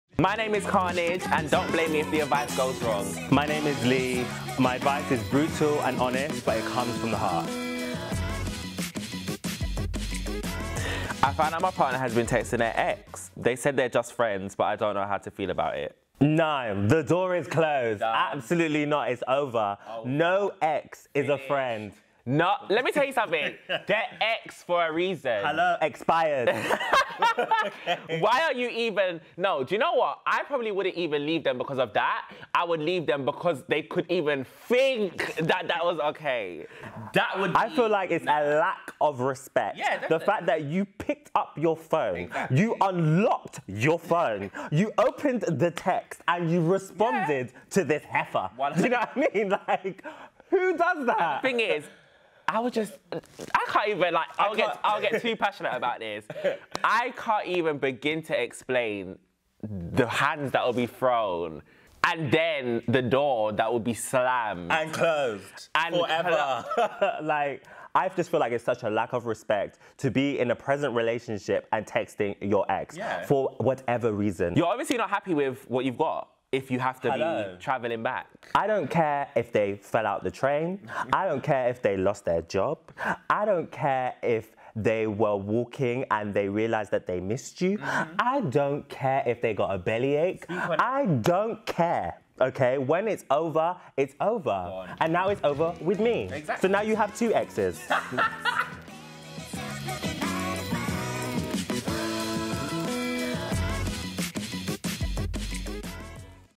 Ask the Aunties: Is texting your ex cheating?
How would you feel if you found out your partner was texting their ex? Does texting your ex count as cheating? This episode's dilemma says: “I found out my partner has been texting their ex. They say they’re just friends but I don’t know how to feel about it."